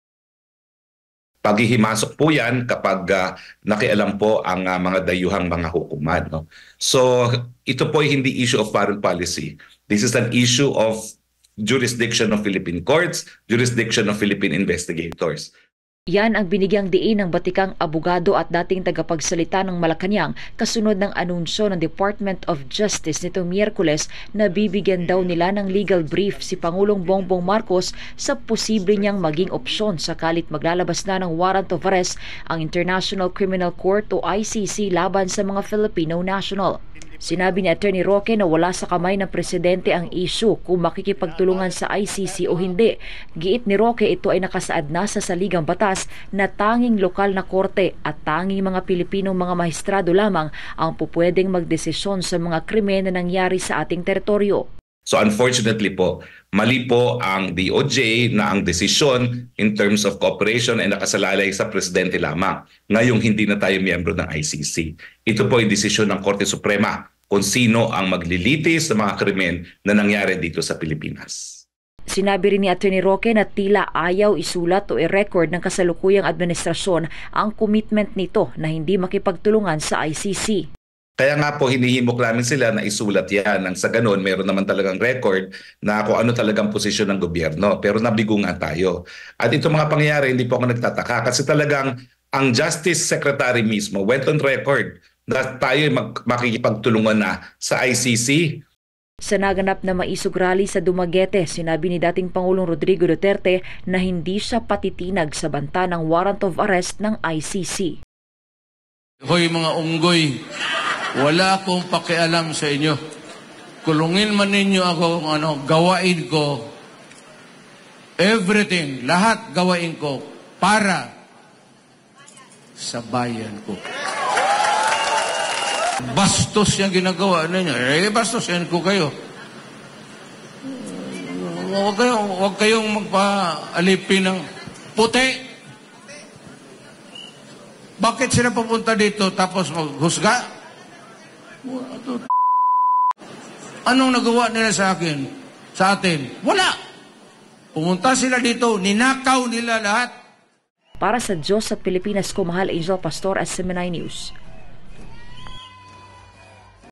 Atty. Harry Roque: Pakikipagtulungan o hindi sa ICC, wala sa kamay ni PBBM
Iginiit ng batikang abogado na hindi lang nakasalalay sa presidente ang desisyon ng kooperasyon ng Pilipinas sakaling maglabas ng warrant ang International Criminal Court (ICC) laban sa mga Filipino national.
Sabi ni Atty. Harry Roque desisyon ng Korte Suprema kung sino ang maglilitis sa mga krimen na nangyari dito sa Pilipinas.